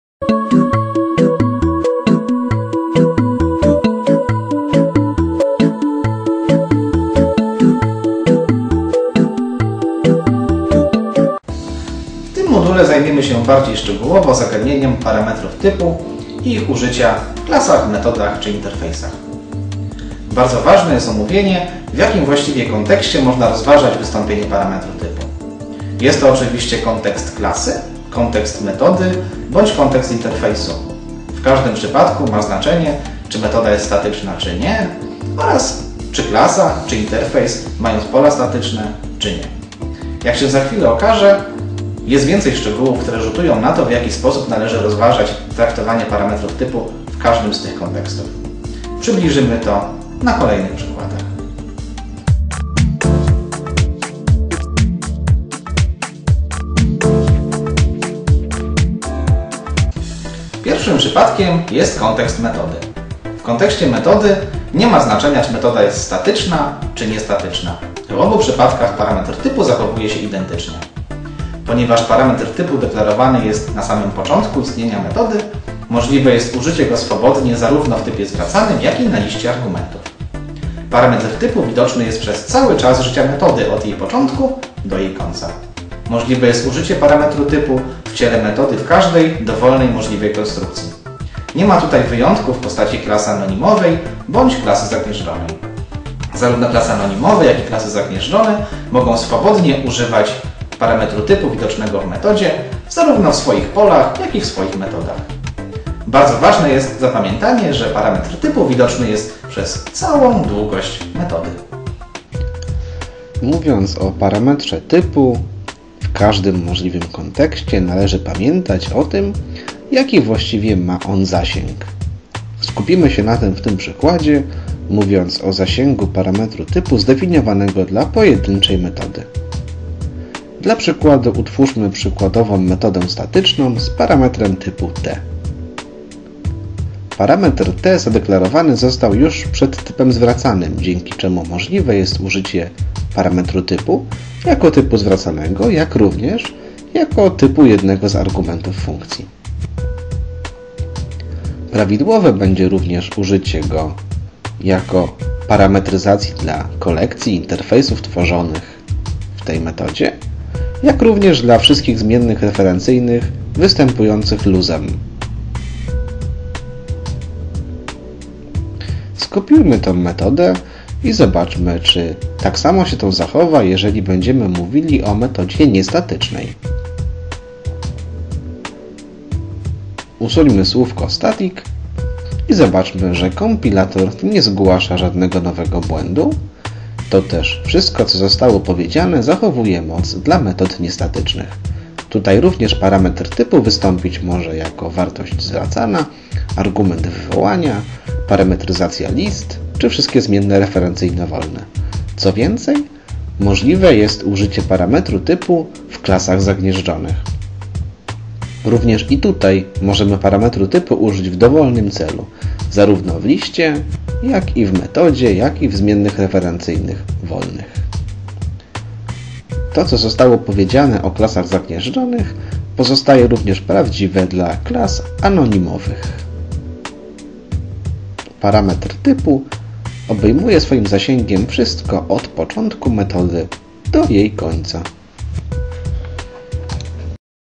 Typy generyczne w języku Java / Moduł 5. Parametry a dziedziczenie, zasięg, kontekst / Cz. 1
Opis modułu:
W module są omówione szczegółowo zagadnienia widoczności i zasięgu parametru typu w kontekście metod, klas i interfejsów. W module szczególny nacisk położony jest na znaczenie kontekstu statycznego dla możliwości użycia parametru typu. Moduł porusza również zagadnienia dziedziczenia po typach parametryzowanych z użyciem parametru typu.

Opis szkolenia:
Devcast "Zaawansowane wykorzystanie typów generycznych w języku Java" zawiera treści związane z wykorzystywaniem i tworzeniem typów generycznych. Na początku są omówione podstawowe mechanizmy użycia, szczególnie w kontekście kolekcji języka Java. Następnie, przechodzimy do bardziej zaawansowanych zagadnień, takich jak refleksja, metody clone i equals, czy też interfejsy generyczne. Przedstawiamy także zagadnienia, które są specyficzne dla języka Java w kontekście typów generycznych, takich jak wymazywanie typów.

Dla kogo: programiści.
Wymagana wiedza i umiejętności: doświadczenie w programowaniu w języku Java
Poziom szkolenia: zaawansowany.

Szkolenie "Zaawansowane wykorzystanie typów generycznych w języku Java" przeznaczone jest dla programistów, którzy bardzo dobrze posługują się językiem Java i chcą szczegółowo zgłębić specjalistyczny temat wykorzystywania typów generycznych w codziennej praktyce programistycznej.

Typy generyczne w języku Java / Advanced use of generic types in Java / Moduł 5. Parametry typu a dziedziczenie, zasięg, kontekst statyczny / DevCastZone